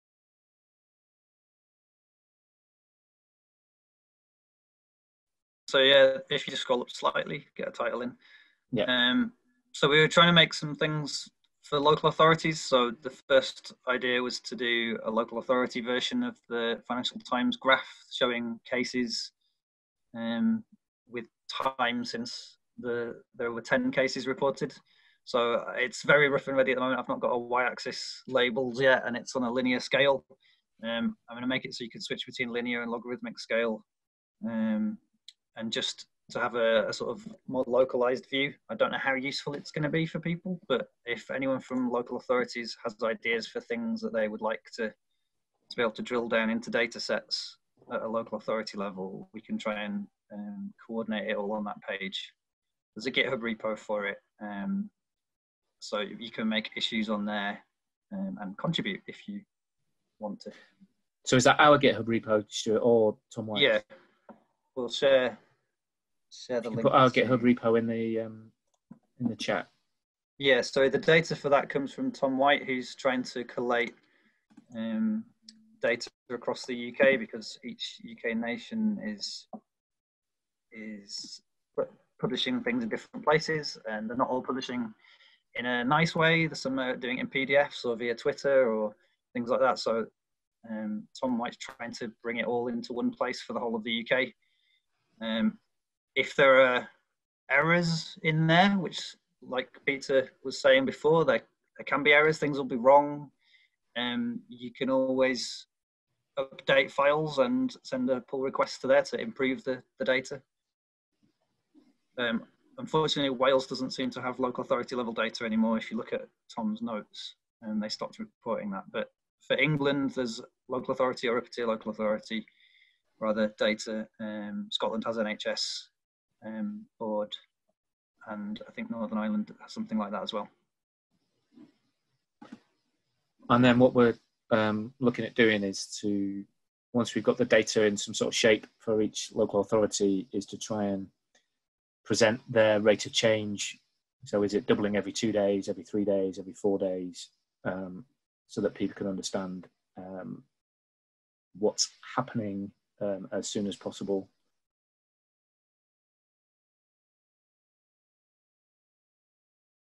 OpenDataSavesLives - Stuart Lowe
Stuart Lowe - ODI Leeds and Open Data Prototype

Originally recorded 2nd May 2020

OpenDataSavesLives is being created to drive open innovation with data. Our aim is to help reduce health inequalities by creating, for the first time, a true network of collaborators and resources in the open.